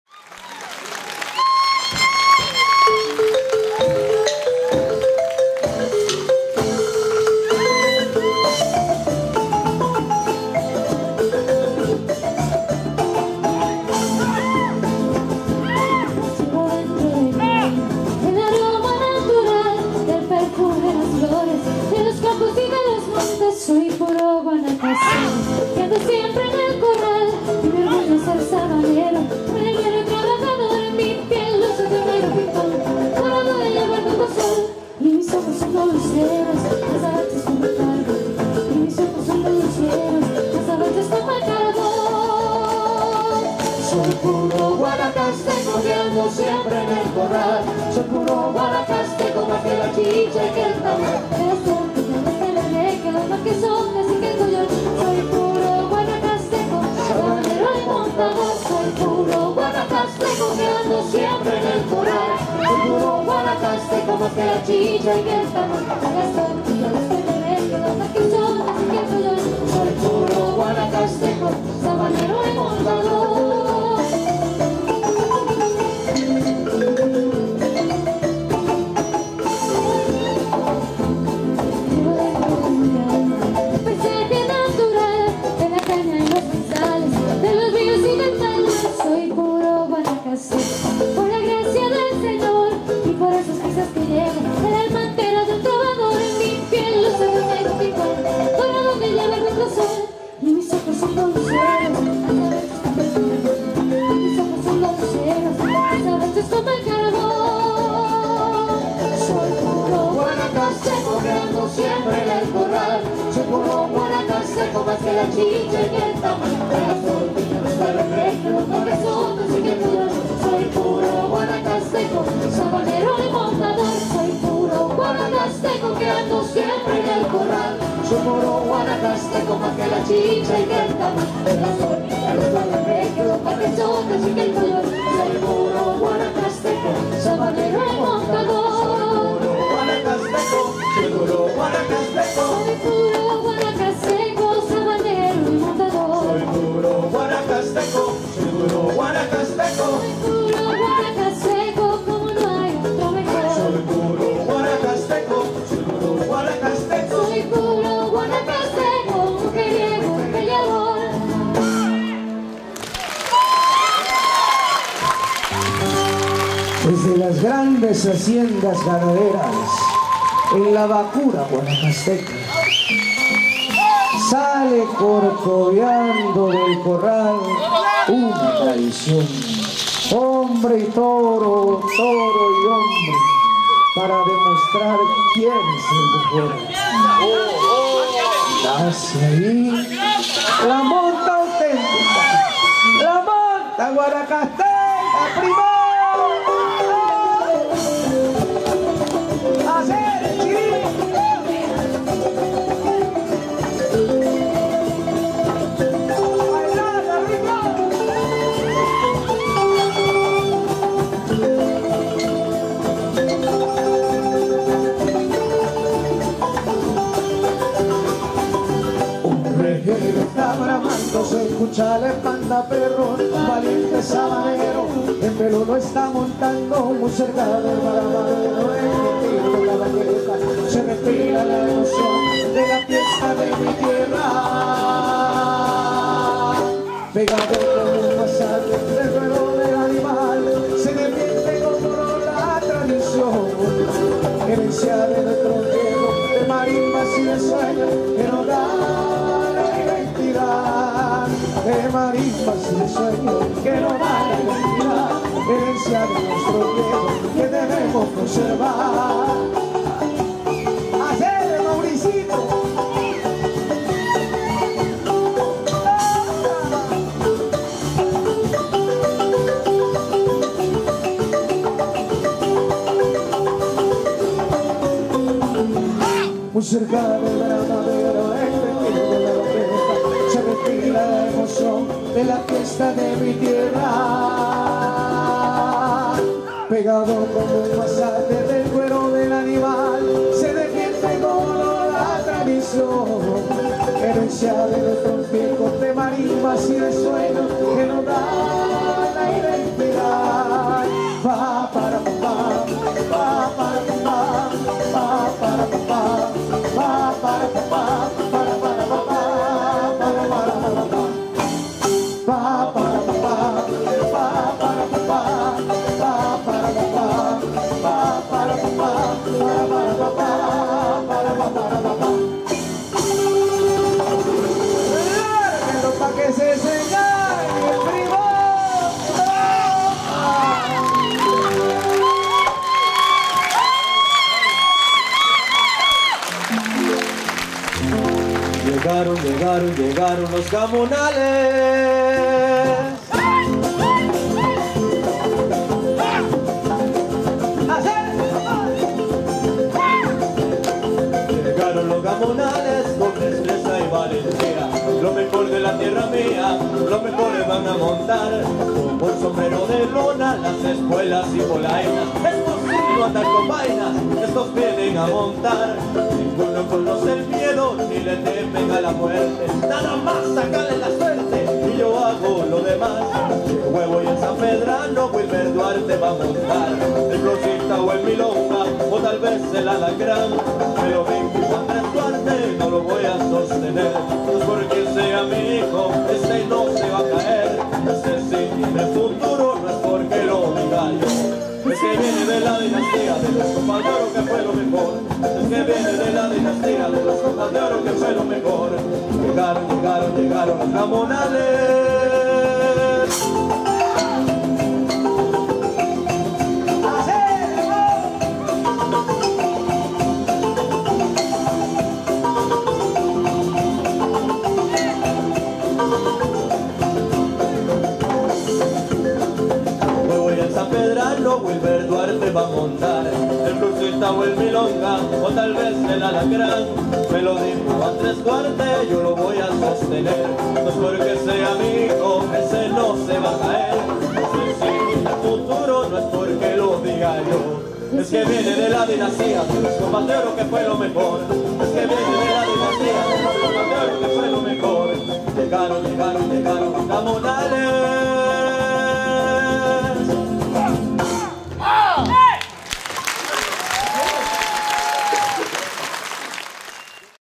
Costa Rica dances from San José
Danza Folclórica Nahualt - San José - Costa Rica.
Summerfest International Folklore Festival and Folk Art Fair Hungary 2016.
XXIII. SUMMERFEST Százhalombatta, Ráckeve, Tököl
Nemzetközi Folklórfesztivál és Népművészeti Vásár.
Ráckeve, Ács Károly Művelődési Központ szabadtéri színpada.   2016. augusztus 17.